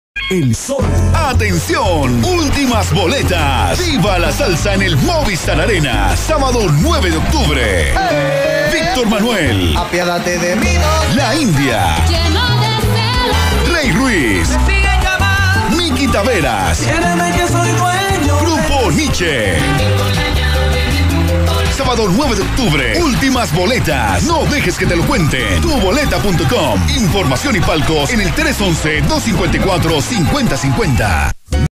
Ultimas Entradas Para Viva La Salsa 2 Movistar Arena Bogotá
Boletería para público general Viva la salsa 2 👉 informe en tuboleta 😍🤩❤️
Estamos listos para ver en vivo a Mickey Taveras, Rey Ruiz, La india, Victor Manuelle y el majestuoso Grupo Niche 😍❤️🤩